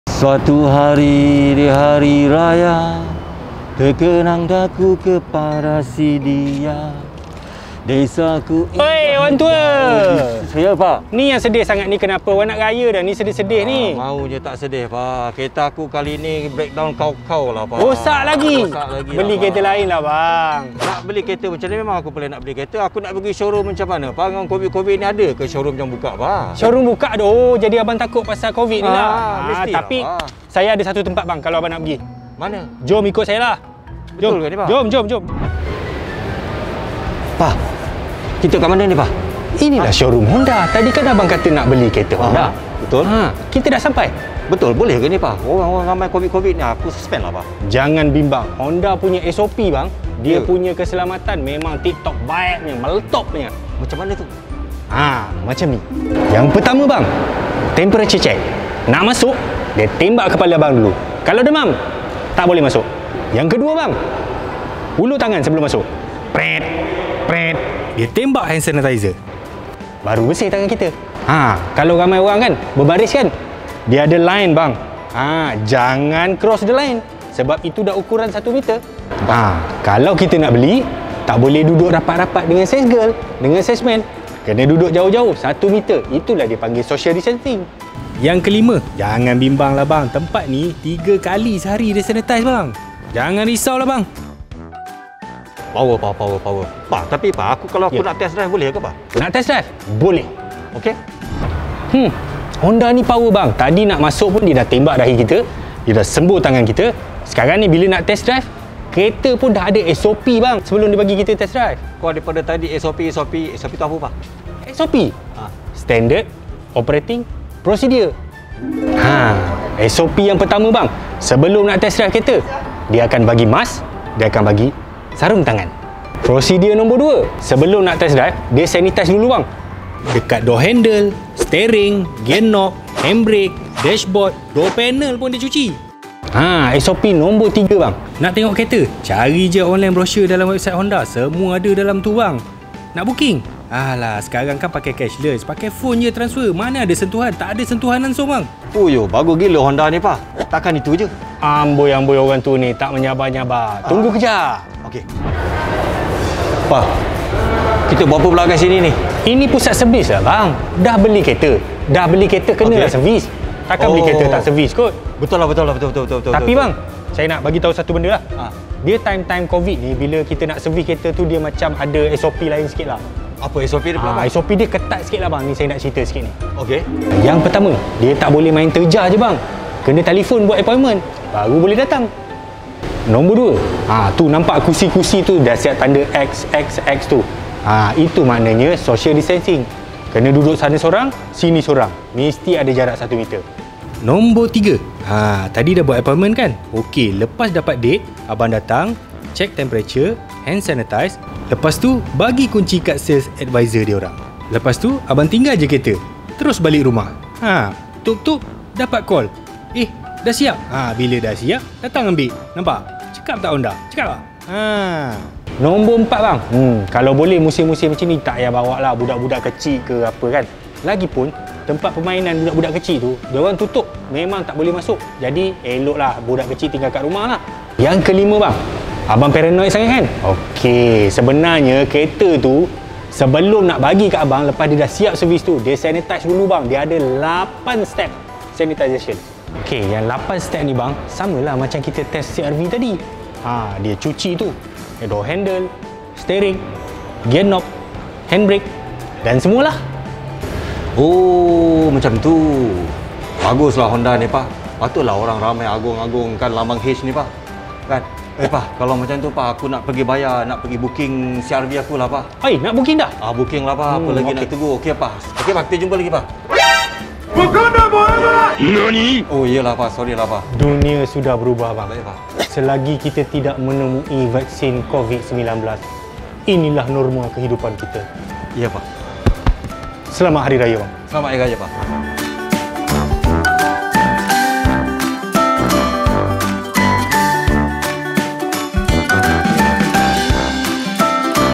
BELI KERETA MASA COVID19? SELAMAT KE?
Abang Beras dok sibuk kata nak angkat kereta satu sempena Aidilfitri.
Tapi, tulah sekarang kan tengah Covid. Selamat ke?

Follow Mekanika Malaysia